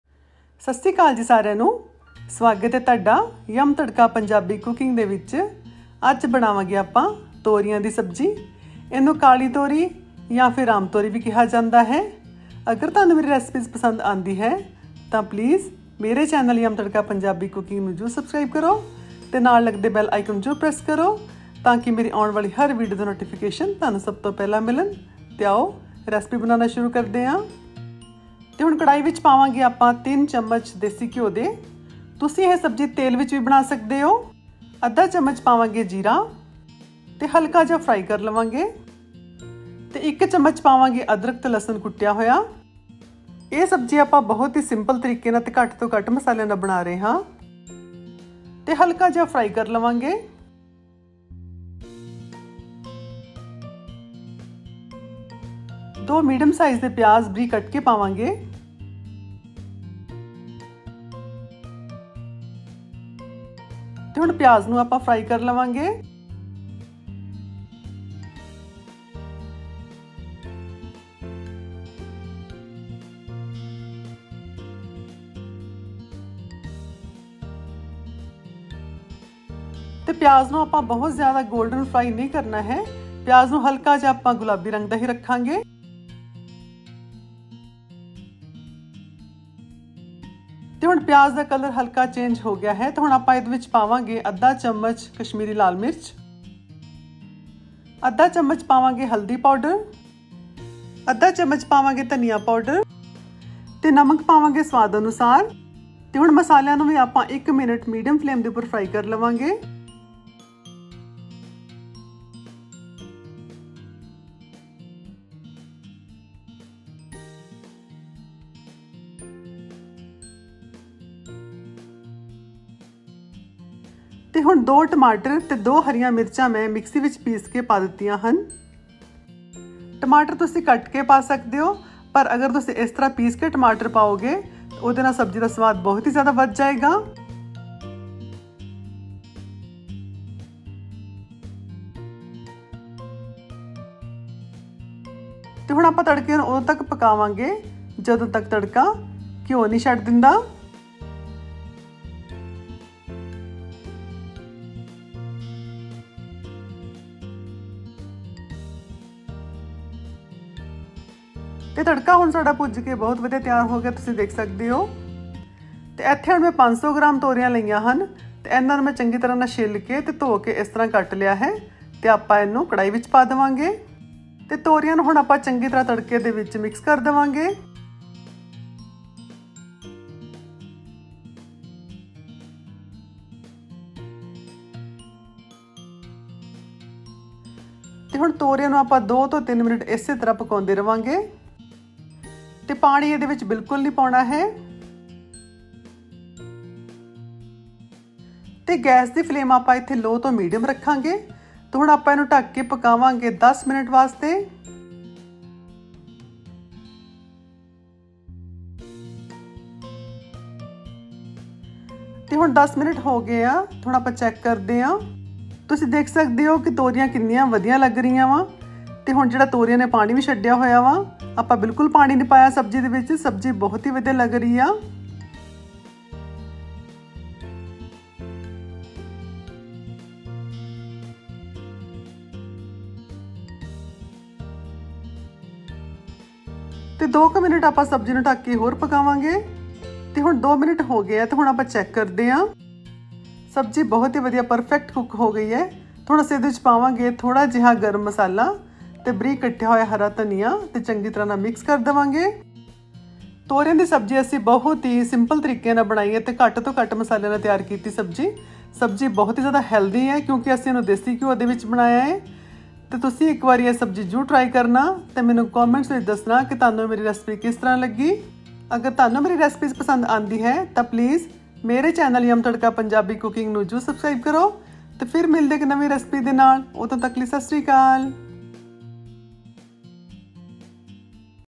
ਤੋਰੀਆਂ ਦੀ ਟੇਸਟੀ ਸਬਜੀ ਬਣਾਓ ਪੰਜਾਬੀ ਤਰੀਕੇ ਨਾਲ ❤️| तुरई की सब्जी बनाए सिर्फ़ 10 मिनट में |Turai Ki Sabji
Ingredients:
Ridge Gourd (turai)-500 gm
Ginger Garlic-1 tbsp, crushed
Clarified Butter(desi ghee)-3 tbsp
Cumin-1/2 tsp
Onion-2 medium, finely chopped
Tomato-2, puree
Green Chilli-2, paste
Kashmiri Red Chilli-1/2 tsp
Turmeric-1/2 tsp
Green Coriander-2 tbsp, finely chopped